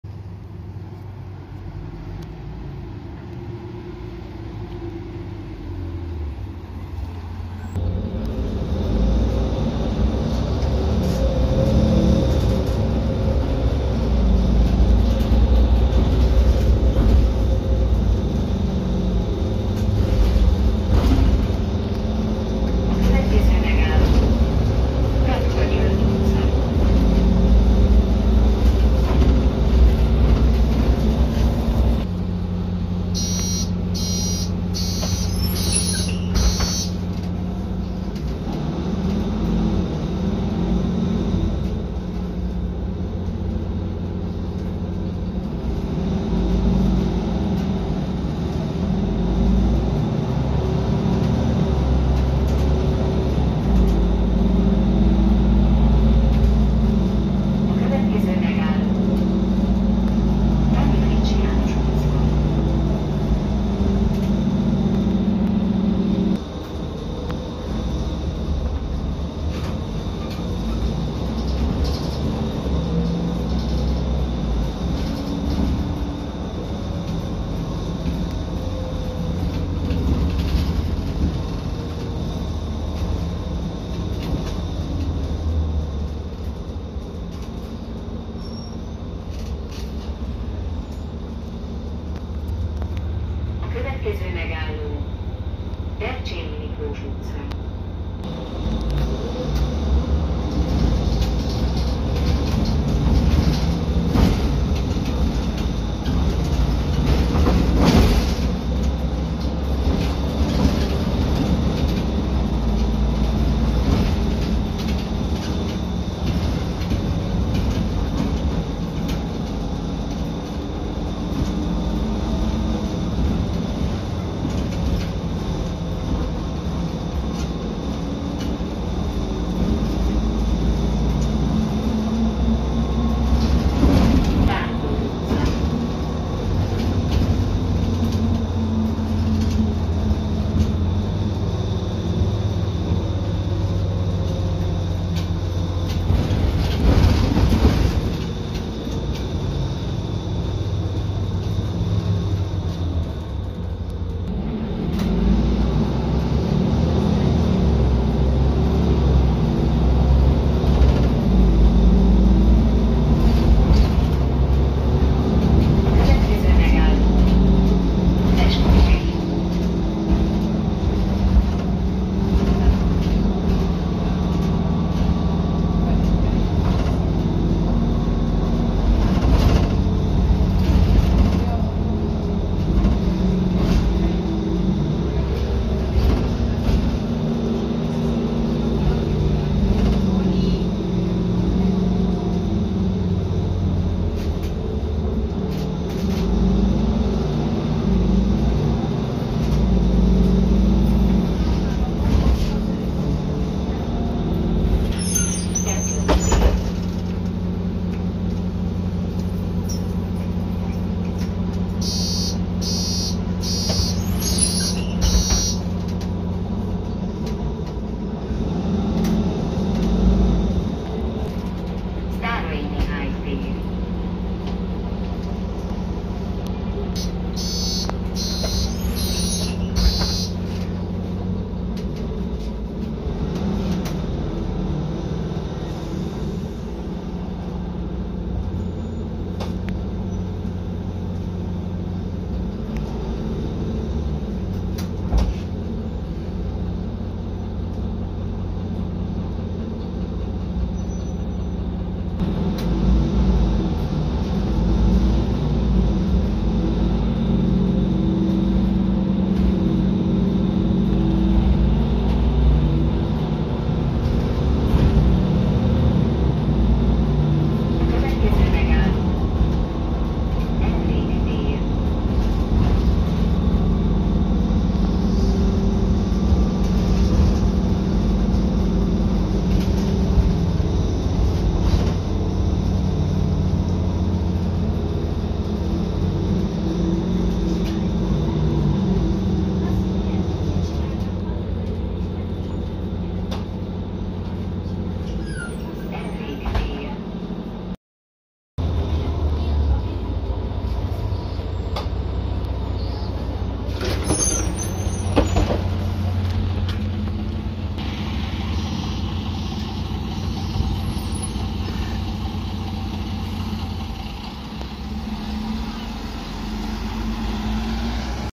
Mercedes Conecto szóló pótol a 124-esen! (PKN-616) RPP. #11
Szerintem eddig ennél a videónál telt el a legtöbb idő a felvétel és a felrakás között, mivel ez a videó JÚNIUS 18-i,viszont valamiért sosem csináltam meg.
Más nagyon nincs, ilyenkor még nagyon sok volt a pótlás, bár eltelt közel egy hónap, és még mindig jópár van, minimum egy-kettő.